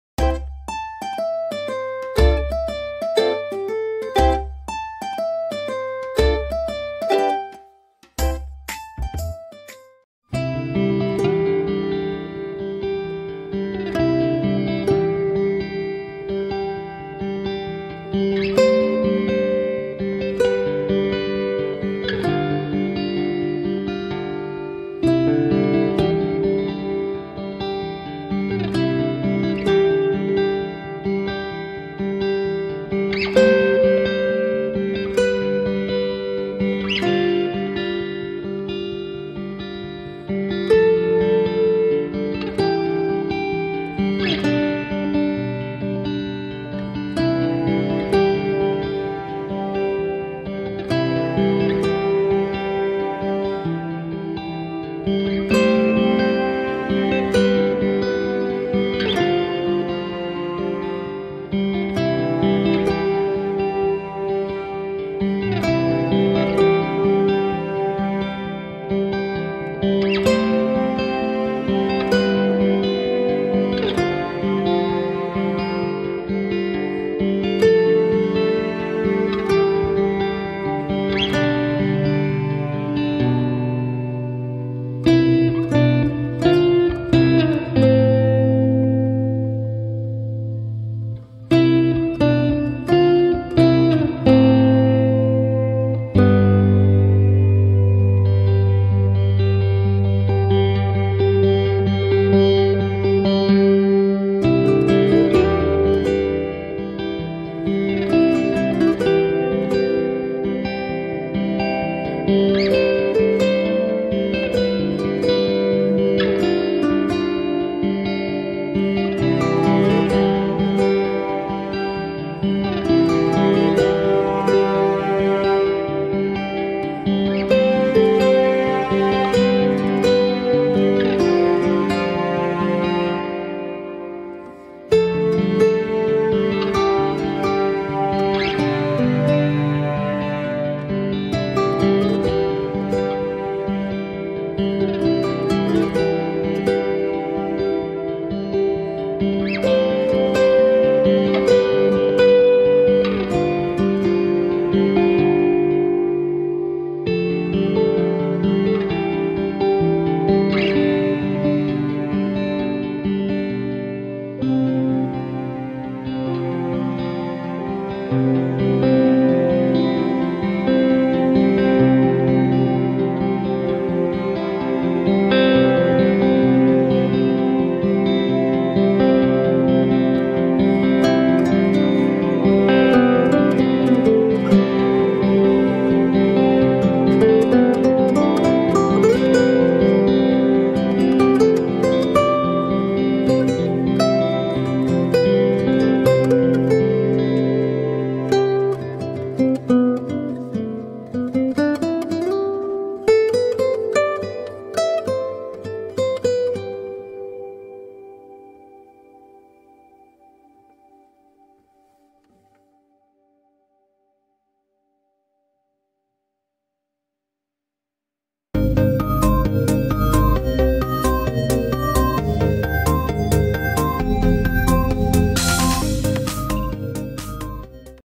Blythe doll coloring book Halloween edition - Cómo montar el libro en miniatura
▼TODA LA INFO AQUI▼

Puedes comprar este libro digital en mi tienda Etsy, listo para descargar al momento e imprimirlo en la comodidad de tu casa. Incluye 15 ilustraciones en A4 y una versión pequeña (escala 1:6) para tus muñecas Blythe.